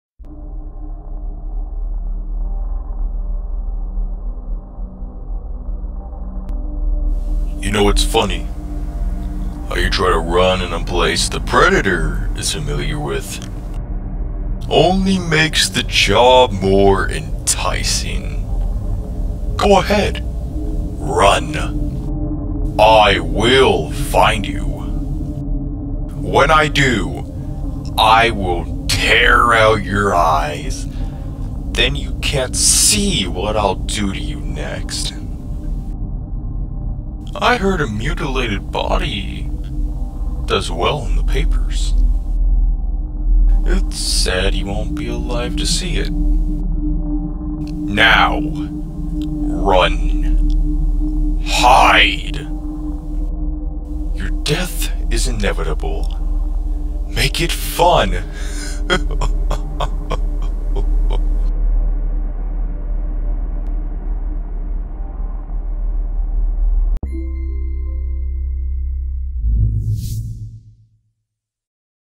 Fanmade Withered Freddy Voice
Yes, this character doesn't have a canon voice so this was a lot easier to do (minus some sound design). I hope you enjoy.